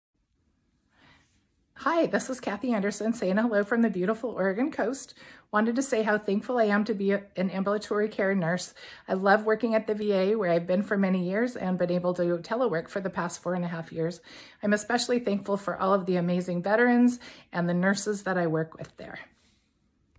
Kathy Andersen
Kathy Andersen tells us why she's proud to be an ambulatory care nurse